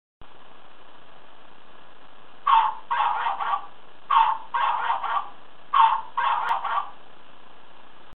Plüschtier Hund Golden Retriever mit Stimme + Sensor 1769
Golden Retriever mit Sensor und Stimme aus Plüsch, ca. 22cm.
für Endkunden oder für B2B.